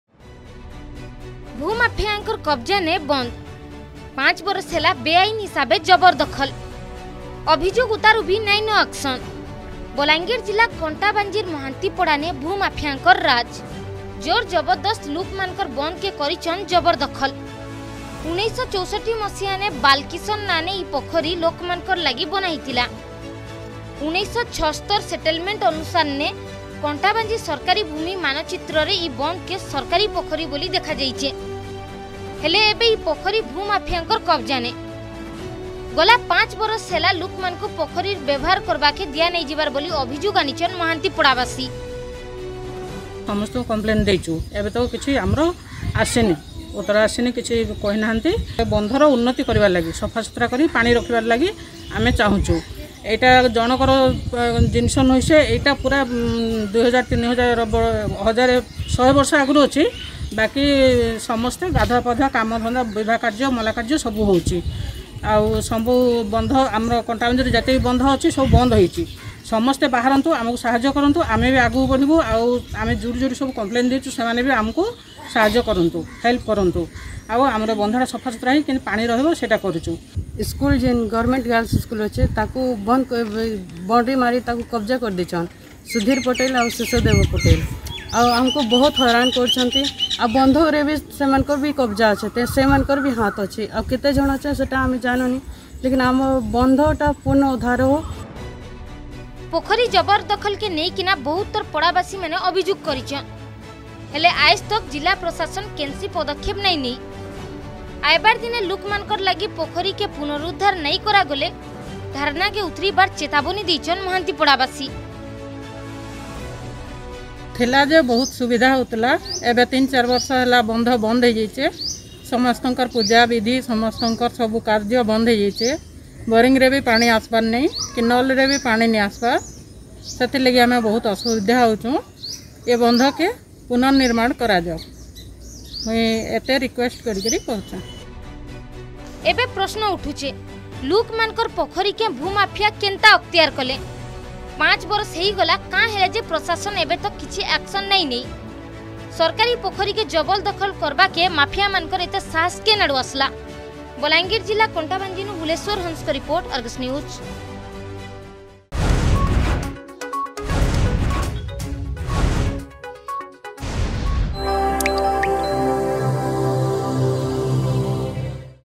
ଭୂ-ମାଫିଆଙ୍କ କବଜ୍ଜାନେ ବନ୍ଧ
5 ବରଷ୍ ହେଲା ବେଆଇନ୍ ହିସାବେ ଜବରଦଖଲ୍ । ଅଭିଯୁଗ୍ ଉତାରୁ ବି ନାହିଁନ ଆକ୍ସନ୍ । ବଲାଙ୍ଗିର ଜିଲ୍ଲା କଣ୍ଟାବାଞ୍ଜି ର୍ ମହାନ୍ତିପଡାନେ ଭୂ-ମାଫିଆଙ୍କର୍ ରାଜ୍ । ଜୋର୍ ଜବରଦସ୍ତ୍ ଲୁକ୍ ମାନକର୍ ବନ୍ଧ କେ କରିଛନ୍ ଜବରଦଖଲ୍ ।